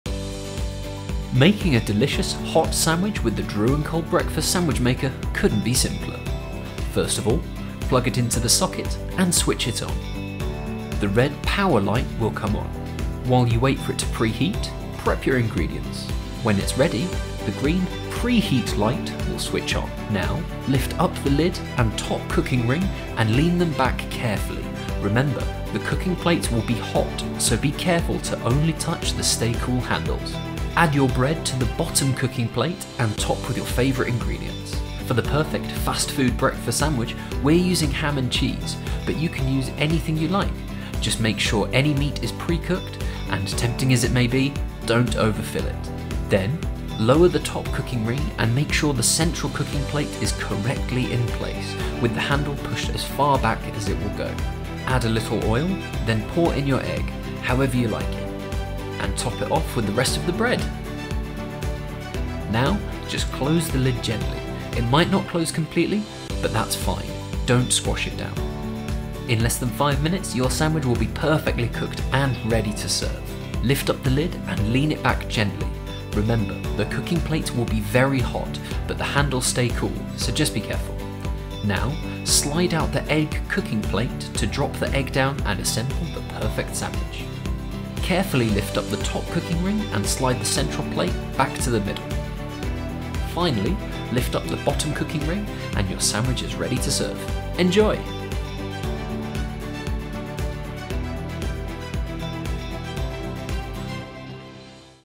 How To Make the Perfect Breakfast Muffin - Drew&Cole Breakfast Sandwich Maker
Make the perfect breakfast sandwich in less than 5 minutes with the Drew&Cole Breakfast Sandwich Maker!